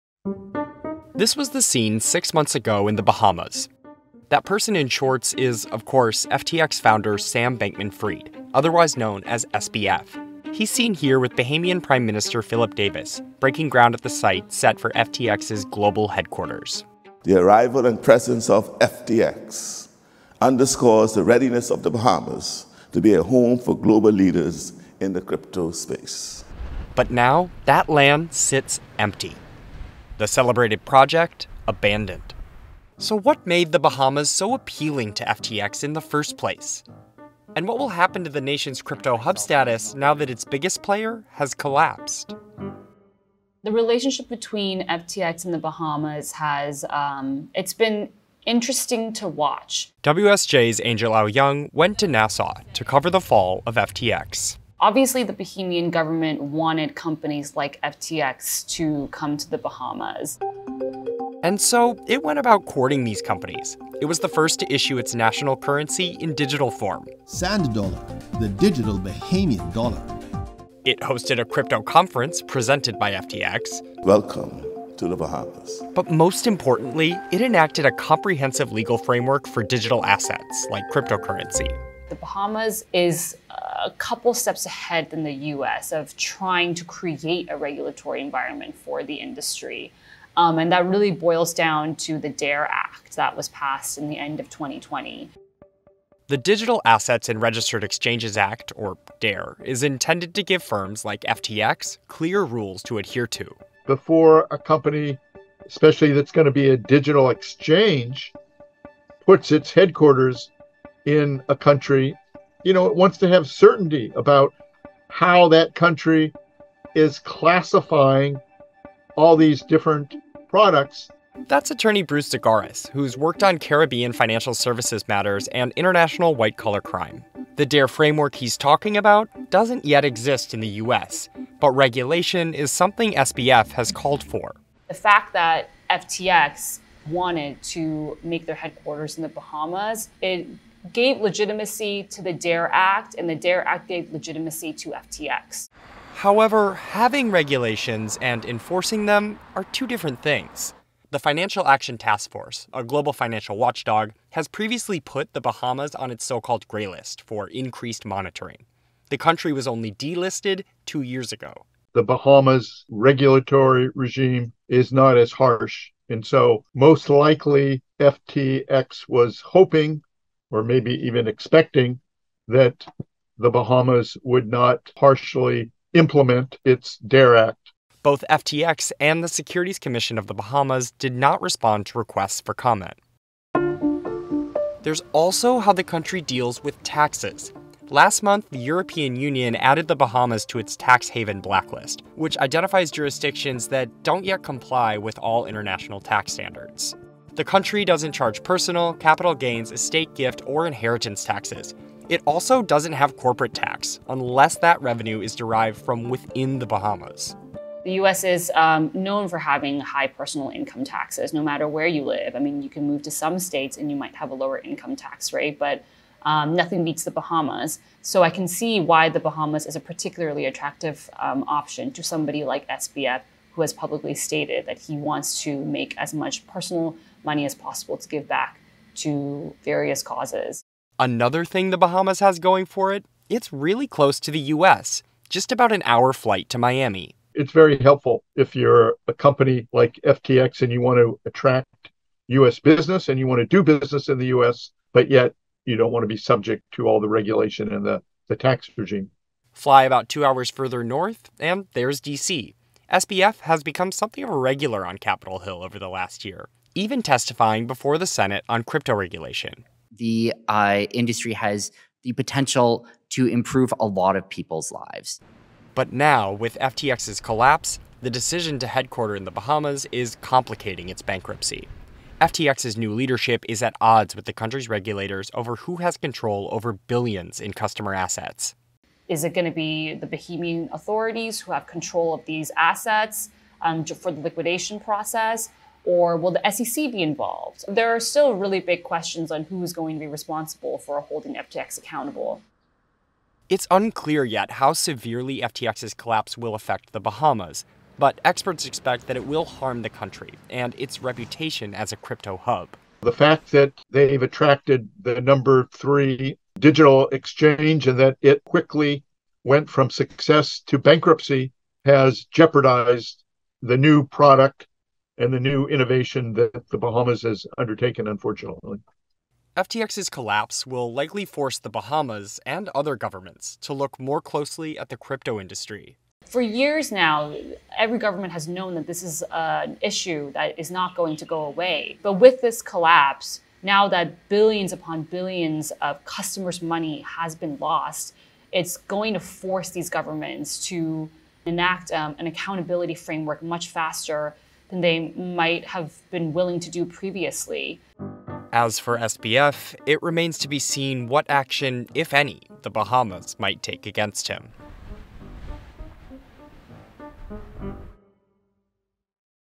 Why FTX Chose the Bahamas for Its Headquarters | WSJ
More than a year before its collapse, FTX moved its headquarters to the Bahamas—a country that worked to lure crypto companies to its shores. So what makes the nation attractive to crypto? And how could FTX’s demise change that?

Illustration: Adele Morgan